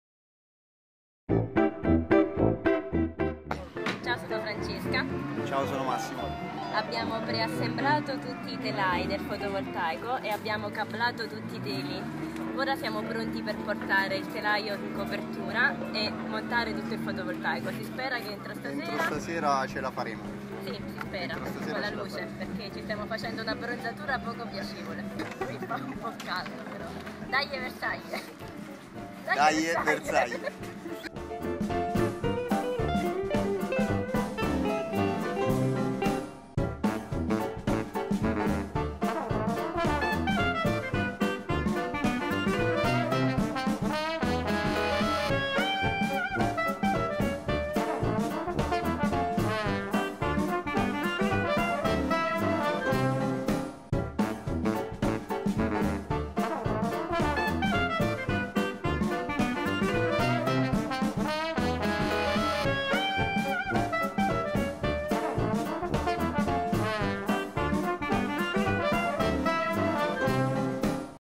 Agenda RhOME - puntata 9 - Il y a du soleil
Oggi è il giorno del sole! Siamo riusciti a finire le finiture del tetto e ora tocca al fotovoltaico.
Massimo e Francesca sono all'opera per messa a punto del sistema integrato che ci permetterà di produrre tanta energia per la competizione.